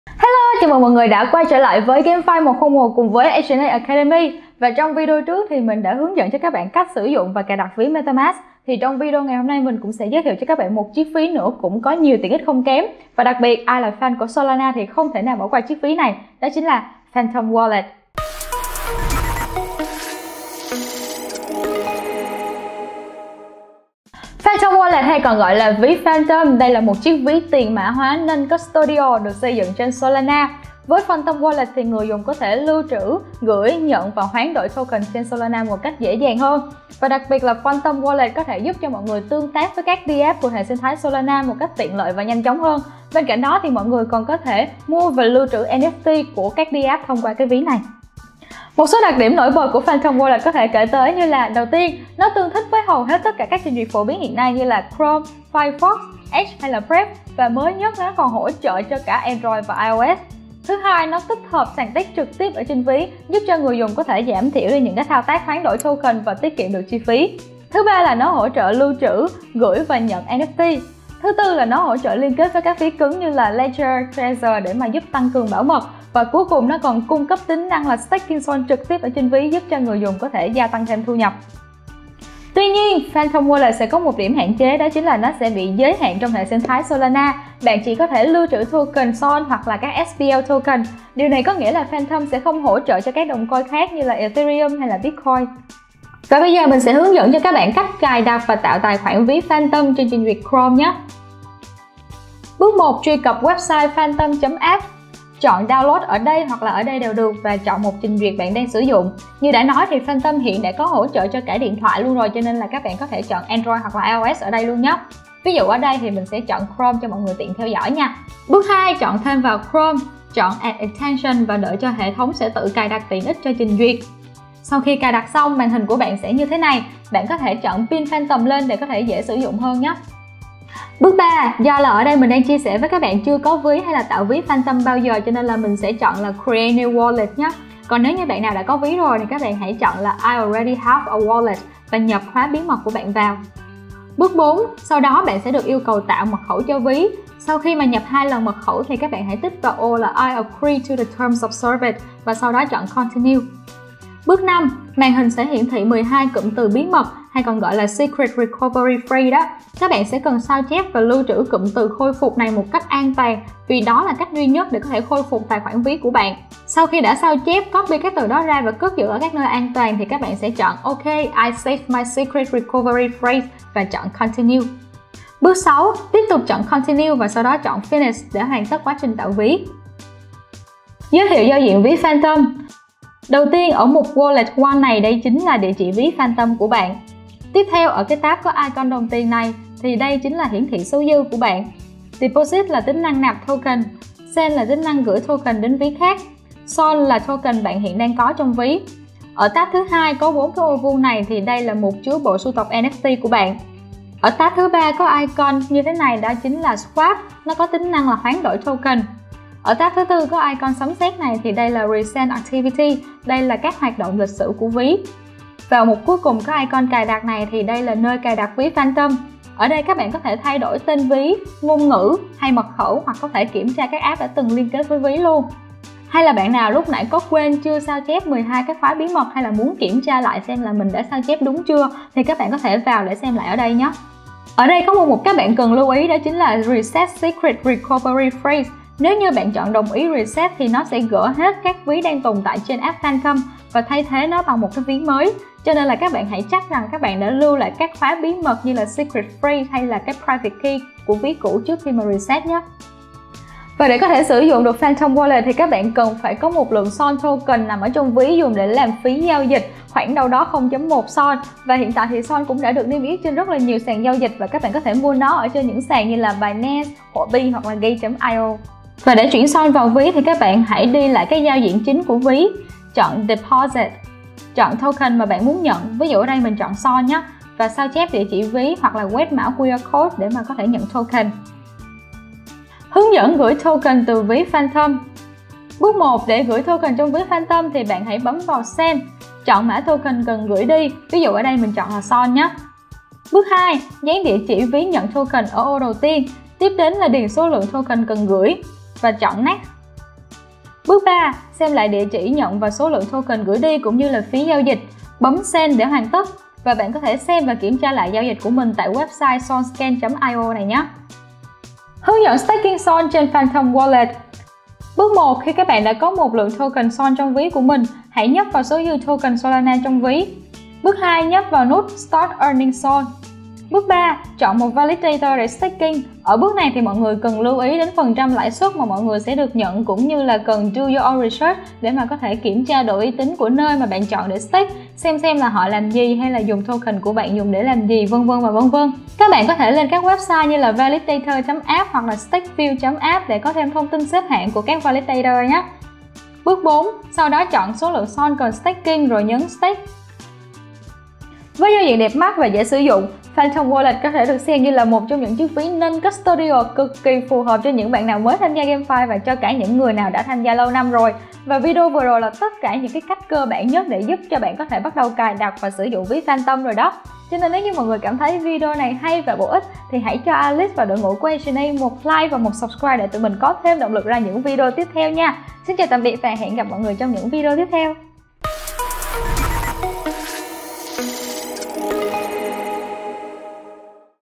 Cách sử dụng VÍ PHANTOM mới nhất - How to use PHANTOM WALLET | GameFi 101 - Ep02 | Ancient8 Academy
Cách sử dụng ví Phantom - How to use Phantom Wallet | GameFi 101 - Ep02 | Ancient8 Academy

Phantom Wallet hay ví Phantom là một ví tiền mã hóa non-custodial, được xây dựng trên Solana. Với Phantom Wallet, người dùng có thể lưu trữ, gửi, nhận token và NFT trên Solana một cách dễ dàng.
Với giao diện đẹp mắt và dễ sử dụng, có thể nói Phantom là 1 lựa chọn ví non-custodial phù hợp với cả người dùng mới và lâu năm.

Cùng tìm hiểu chiếc ví Phantom này trong video của Ancient8 Academy nhé!